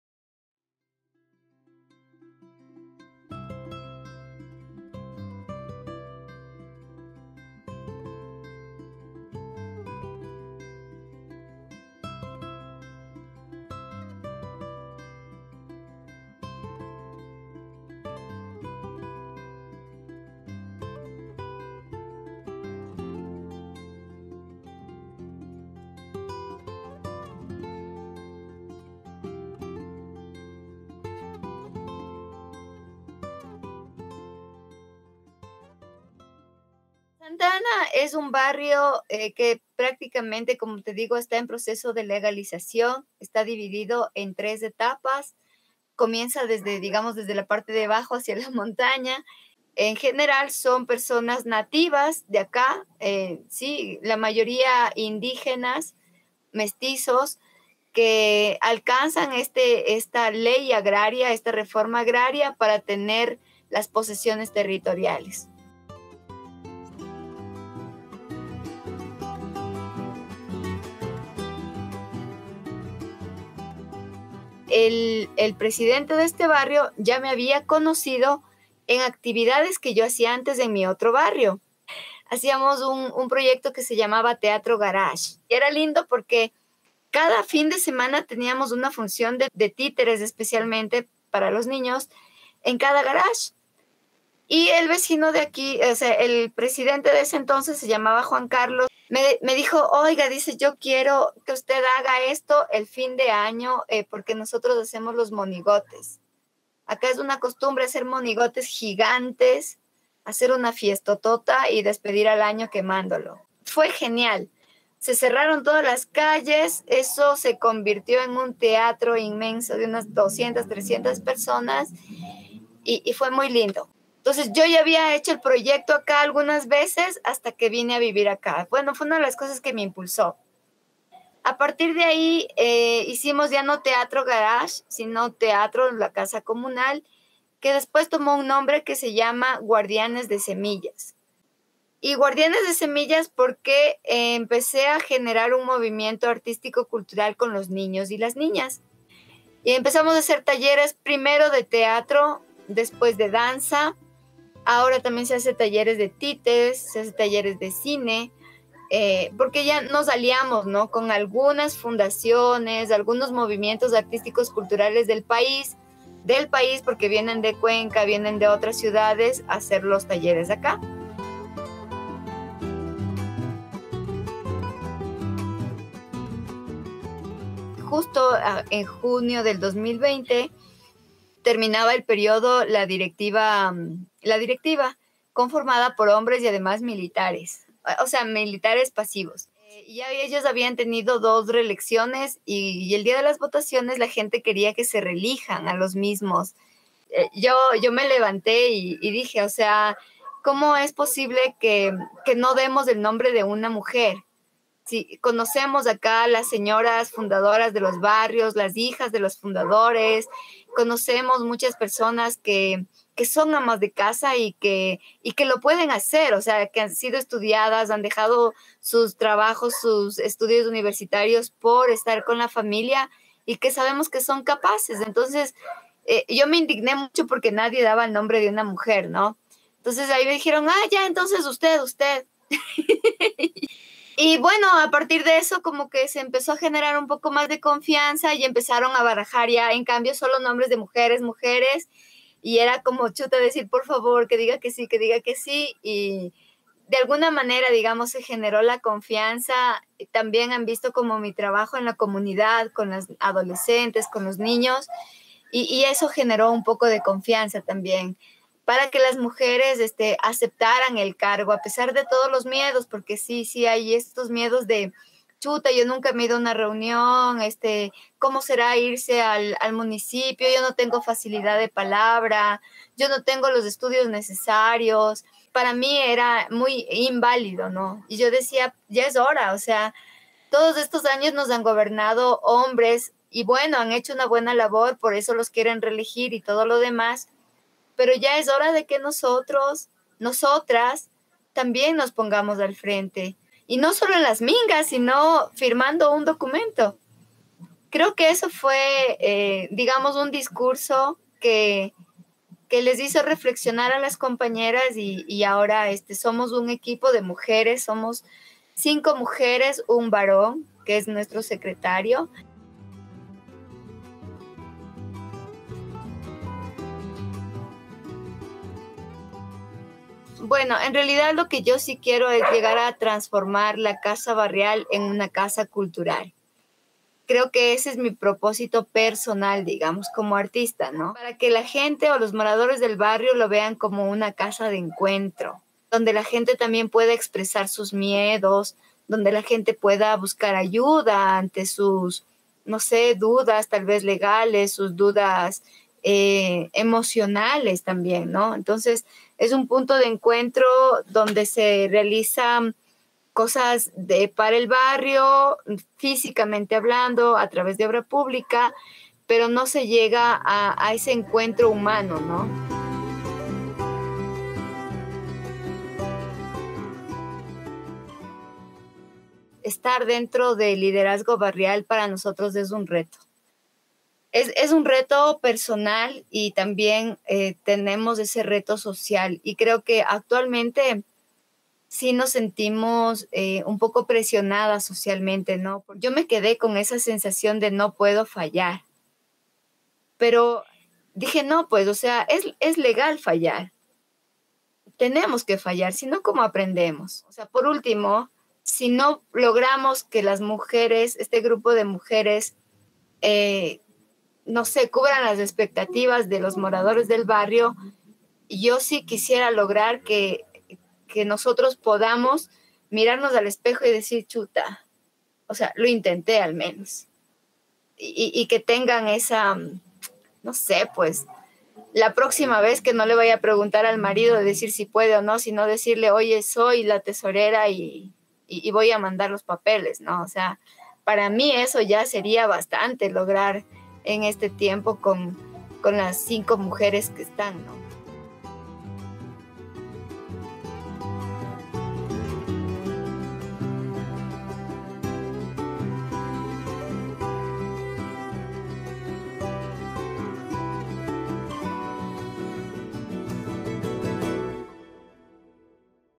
Noemi Laines - Ecuador / Cámara emergente: Mujeres que conectan realidades y territorios
En esta serie multimedial de relatos a nivel de Latinoamérica, entregaremos historias de mujeres que han construido territorio y desde sus particularidades y posibilidades conectan historias sobre  memoria, cuidado, el poder de lo colectivo y la lucha por condiciones dignas.

El barrio Santa Ana Alta del Condado es un barrio periférico, marginal, ubicado junto al bosque protector de Pichincha al noroccidente de Quito. En la ordenanza municipal del 2016 constaba que tenía 20 años de asentamiento y apenas en ese año tuvo reconocimiento legal. Noemi lleva 7 años viviendo en el barrio. Hace parte de la primera organización barrial liderada por mujeres. “Ha sido muy duro llegar a estos espacios” asegura y continúa “porque la misma comunidad no cree que las mujeres podamos liderar”. En el proceso de liderazgo barrial incluyen talleres y eventos para socializar lo artístico pues Noemi hace 11 años es directora de Ukumbi Teatro.

Nos Mueve la Cultura
Comfenalco Antioquia
Alcaldía de Medellín